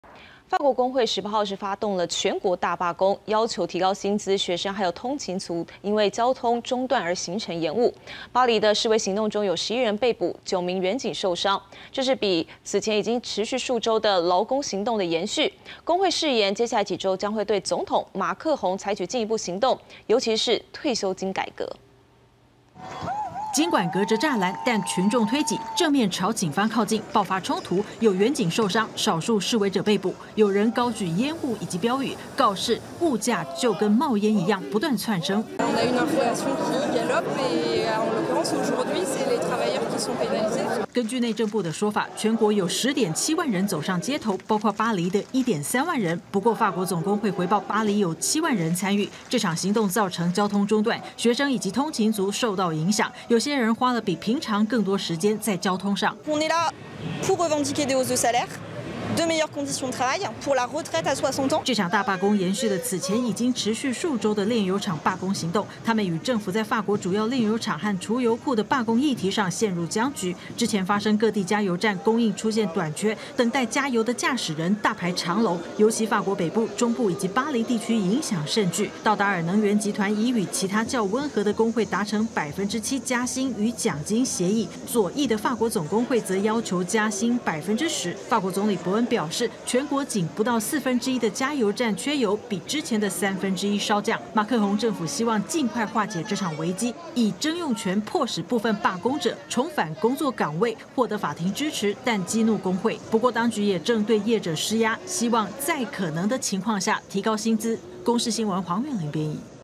法國全國大罷工要求加薪 未來擬對退休金改革續抗爭｜20221019 公視晚間新聞
法國工會18日發動全國大罷工，要求提高薪資，學生及通勤族因交通中斷而行程延誤，巴黎的示威行動中有11人被捕、9名員警受傷，這是此前已經持續數週的勞工行動之延續。工會誓言接下來幾週將對總統馬克宏採取進一步行動，尤其是退休金改革。